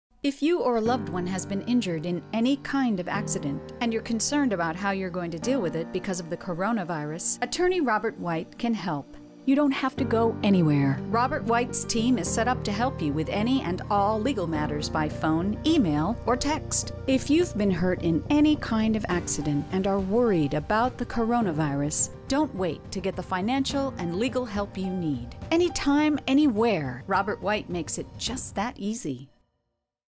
Hurt in an Accident? Attorney Robert White Can Help!
If you've been hurt in an accident and are worried about the coronavirus, you don't have to wait to get the legal help you need.

Attorney Robert White can help you with your claim through phone, email, or text.